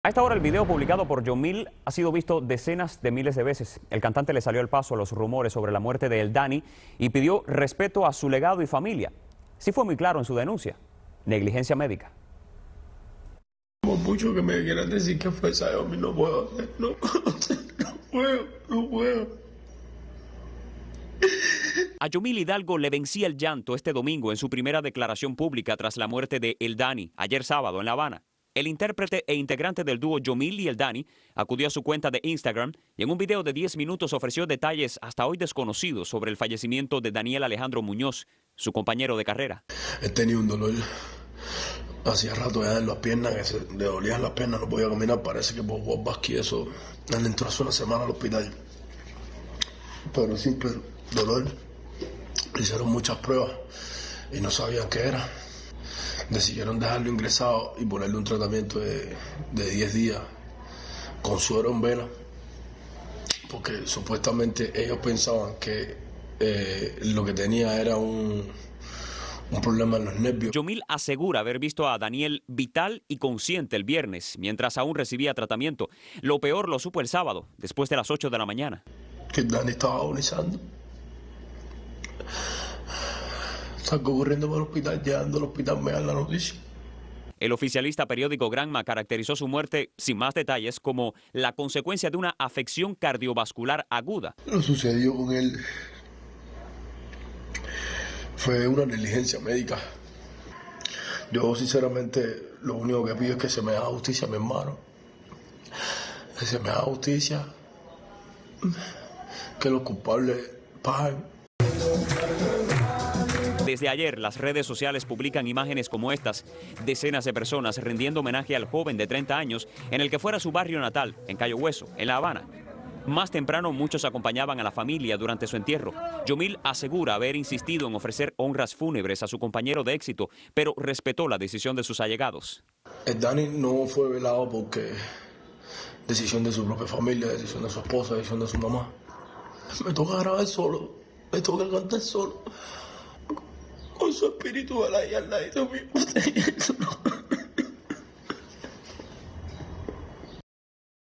Desconsolado Yomil por la muerte de El Dany
Al cantante Yomil Hidalgo le vencía el llanto este domingo en su primera declaración pública tras la muerte de “El Dany” el sábado en La Habana. El integrante del dúo “Yomil y El Dany” acudió a su cuenta de Instagram y, en un video de 10 minutos, ofreció detalles hasta hoy desconocidos sobre el fallecimiento de Daniel Alejandro Muñoz, su compañero de carrera.

“Lo sucedido con él fue una negligencia médica (…) lo único que pido es que se me haga justiciar, que los culpables paguen”, dijo Yomil.